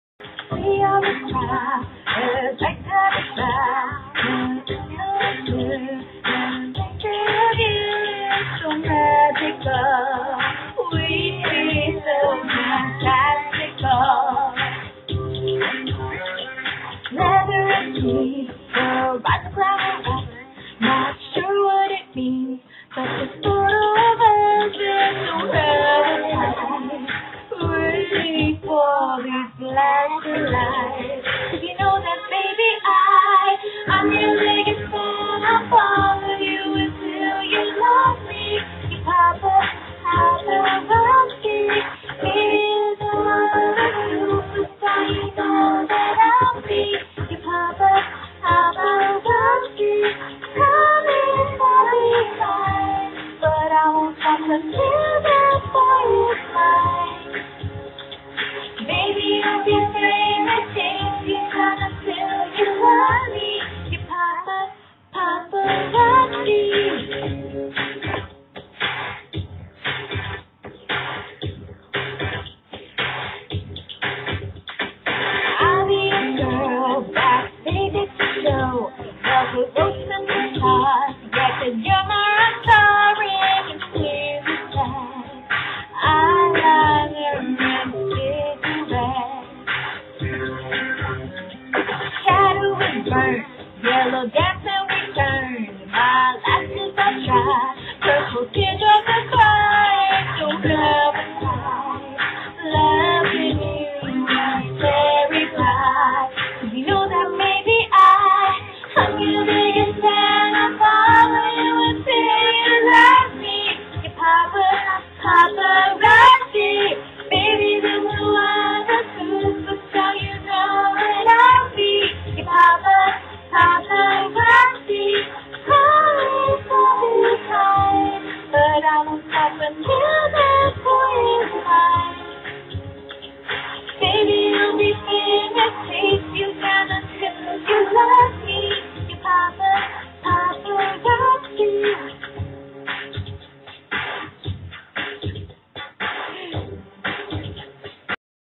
Papparrazi
me and my god daughter havin some funnnn. this is what happens when i'm in charge of baby sitting. hahahah